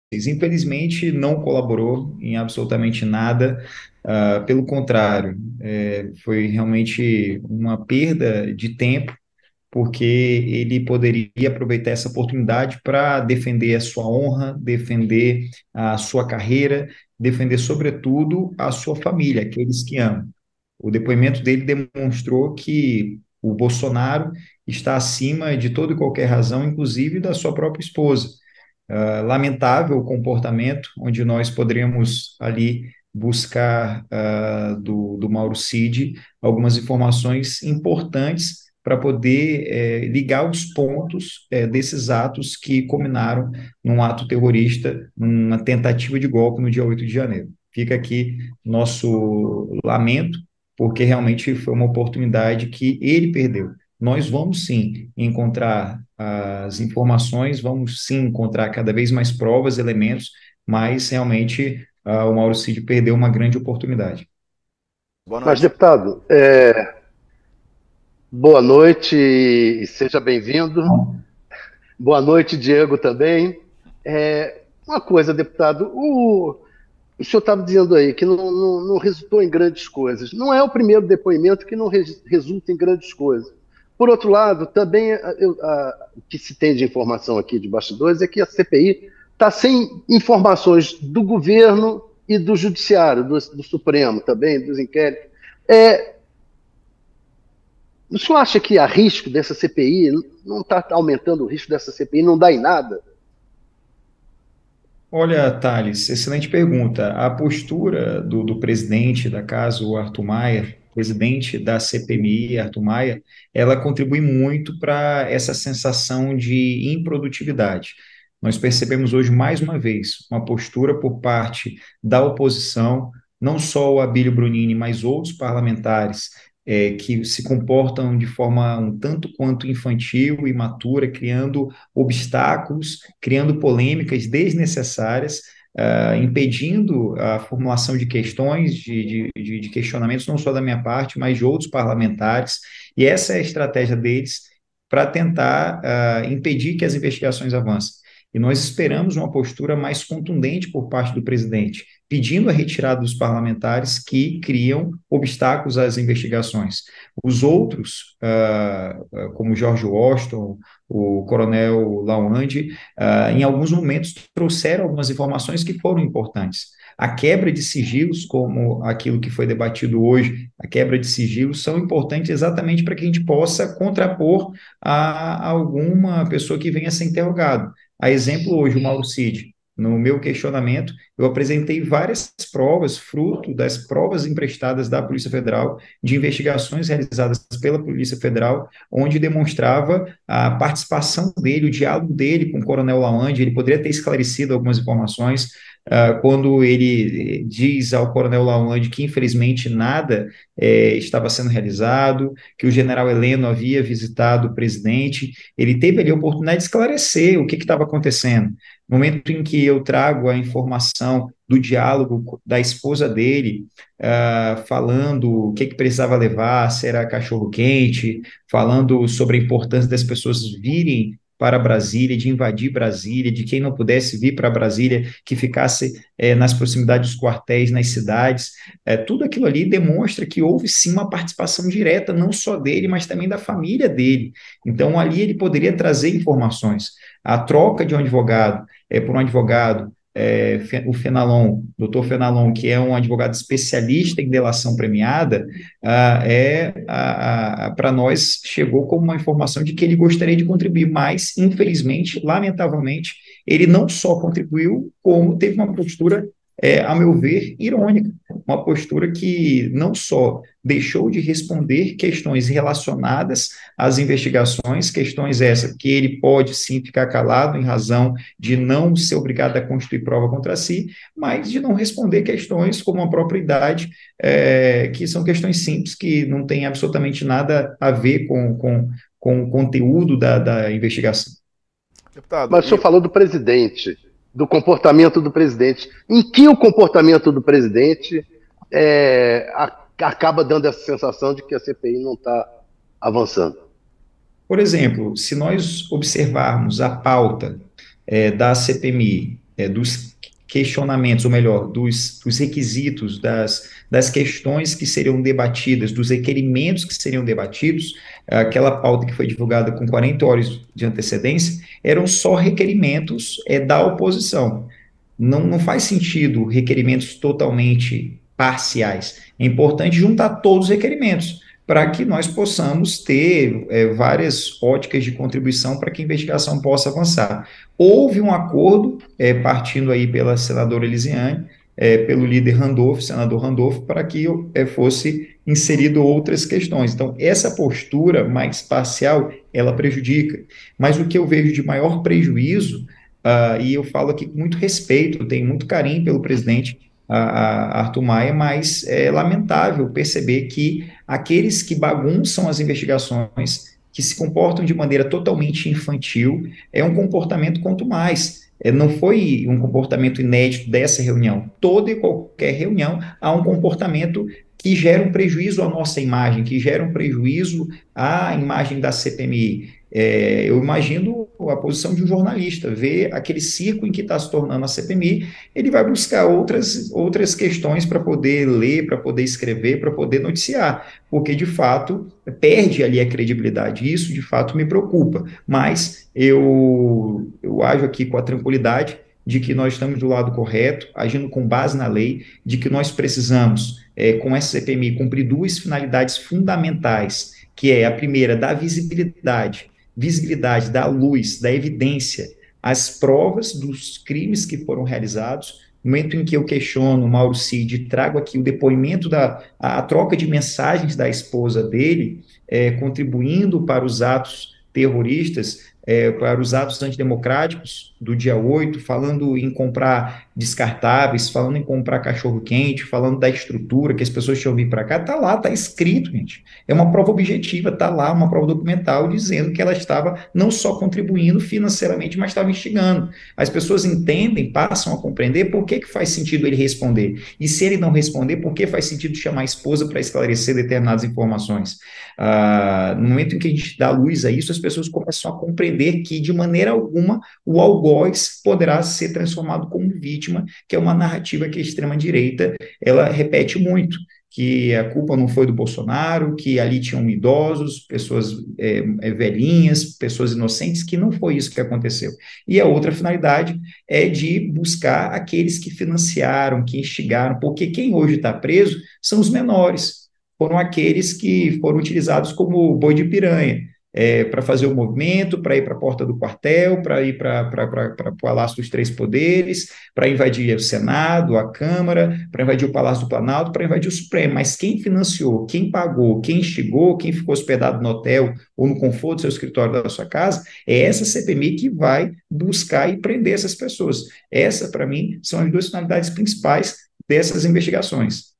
CPI do 8/1: Postura parcial de Arthur Maia contribui para improdutividade da comissão, diz deputado
Em entrevista ao UOL News, o deputado Duarte Jr. comentou os trabalhos da CPI que investiga os atos golpistas de 8 de janeiro.